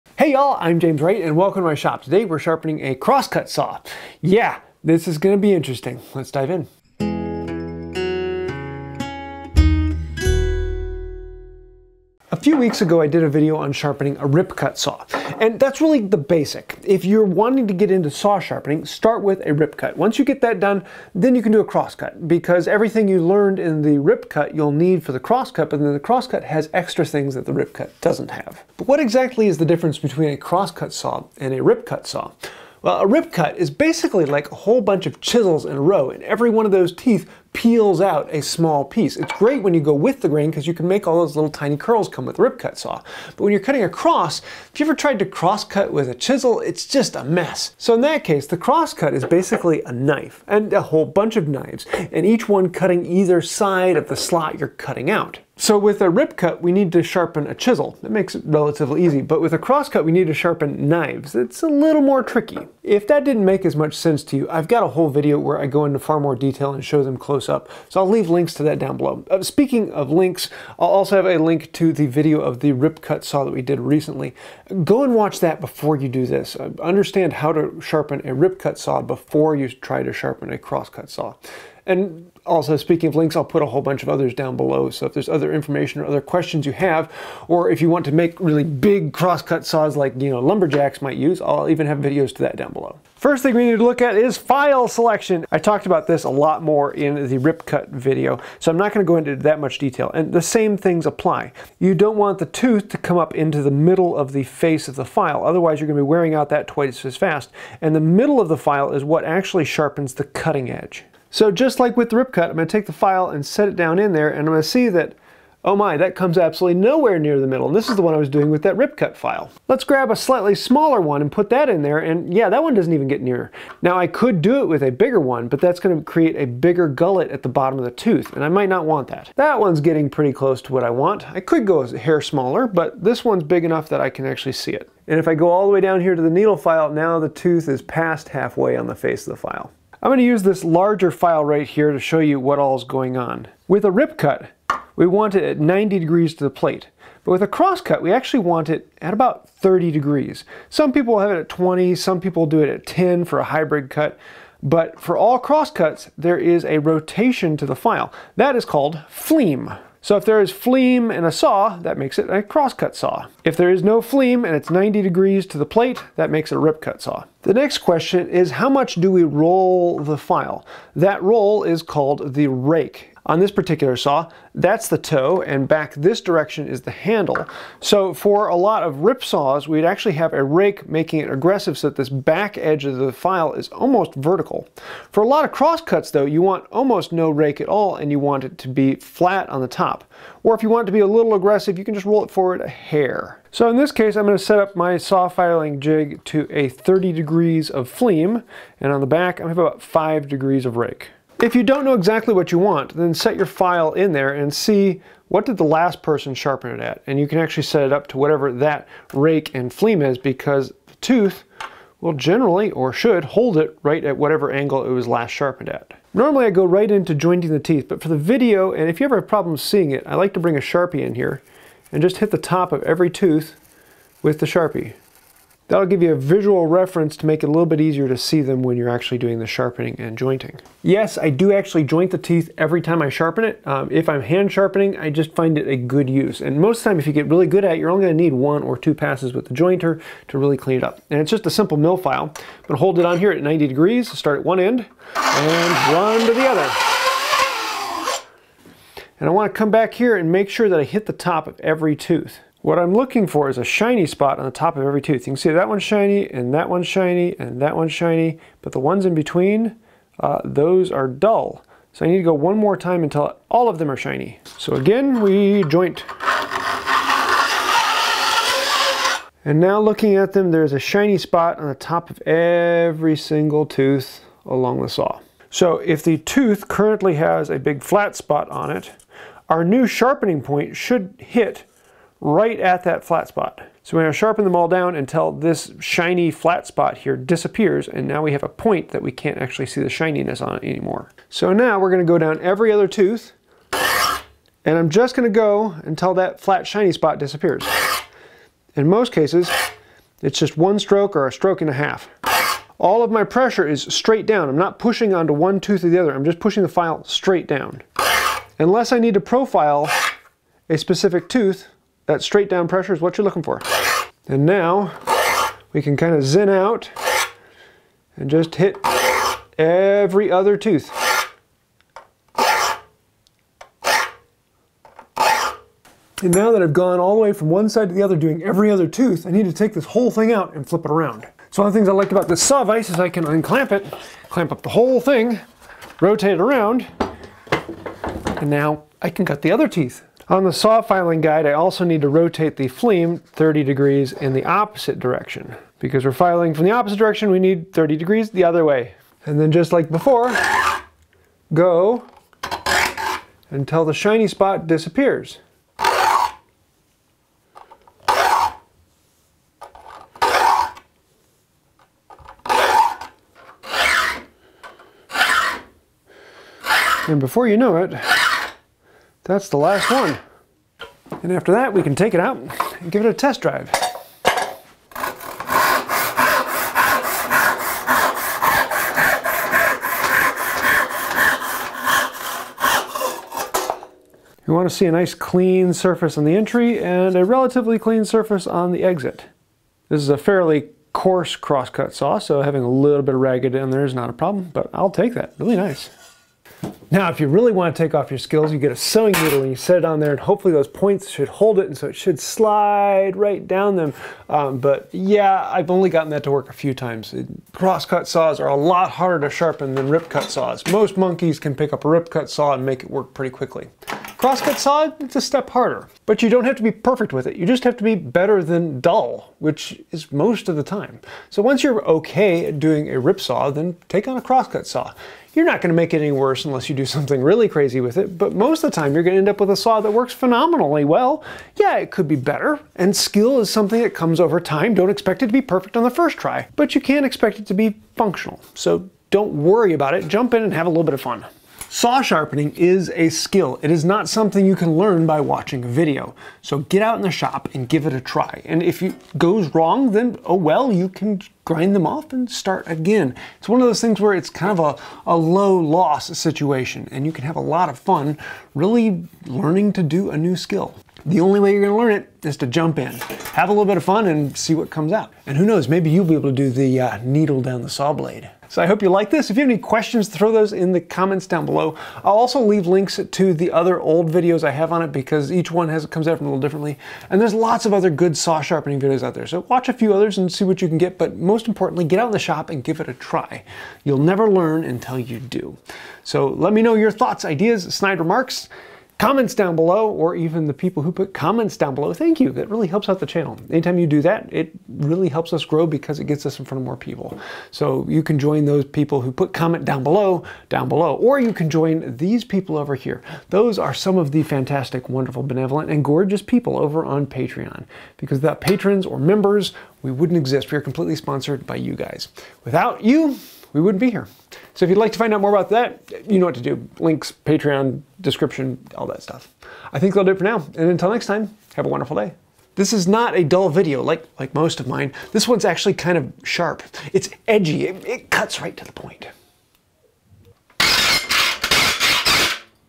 How To Sharpen A Hand Saw Cross Cut Version
Sharpening a crosscut saw is a little more difficult then sharpening a crosscut saw but not that much more dificult.

Miah Beach:

Sherlock Campbell
Andrew Wilson
Brian Suker
Kenny-Anjanette Horn
Russell Gough
Christopher Brown
Ian McElcheran
Brett Lance